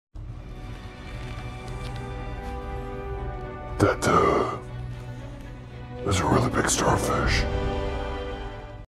Batman Says That's A Really Big Starfish
Just Batman talking about a big alien starfish in this